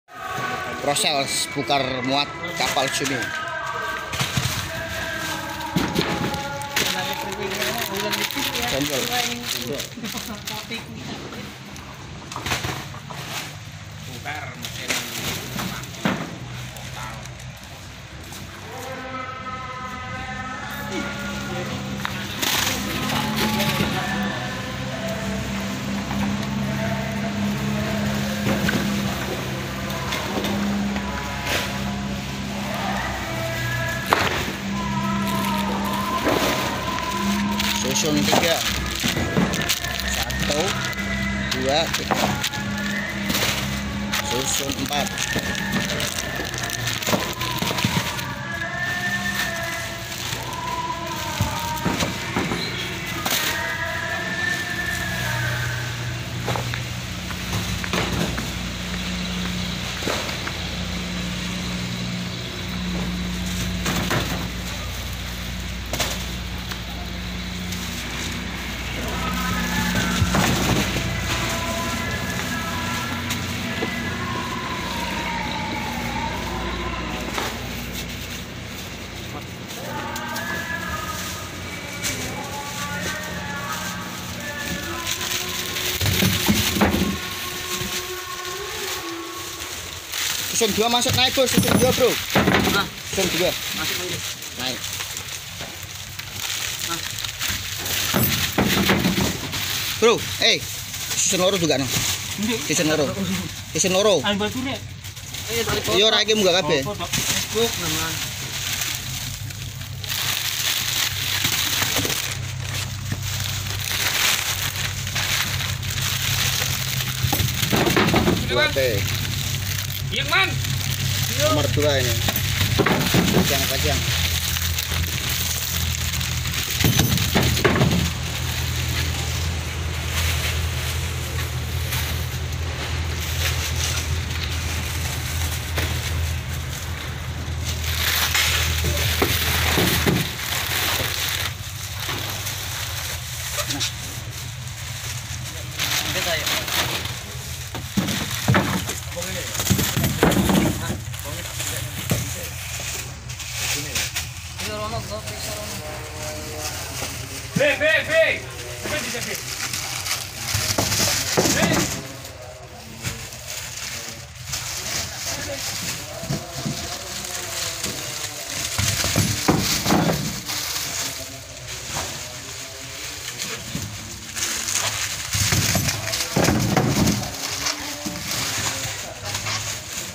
CUMI SOTONG Kapal cumi indonesia
Kapal cumi juwana indonesia ber oprasi pulang pergi 3 bulan dengan anak buah kapal kurang lebih 12 orang dan dengan bagi hasil bukan gaji tapi hasil nya melebihi gaji.
Semoga yang krja di laut selalu dpt lindungan dari allah dan hasil yg berkah...amiin